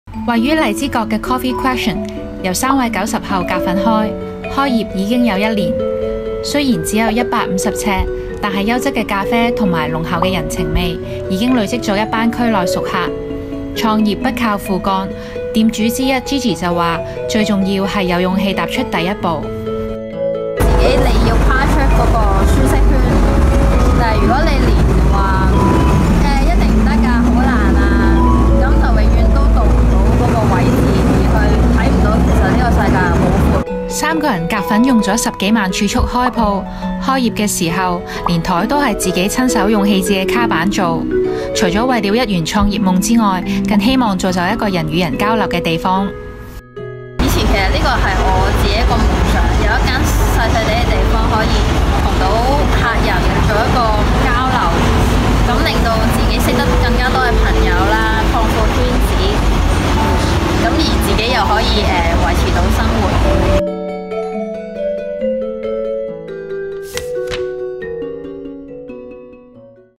90後開150呎Cafe 賣啡也賣人情味
位於荔枝角的Coffee ?由3位90後開設，只有150呎的咖啡室，空間雖小，卻滿載創業初哥的熱誠。開業快滿1年，小舖已累積了一班區內熟客。(更多內容：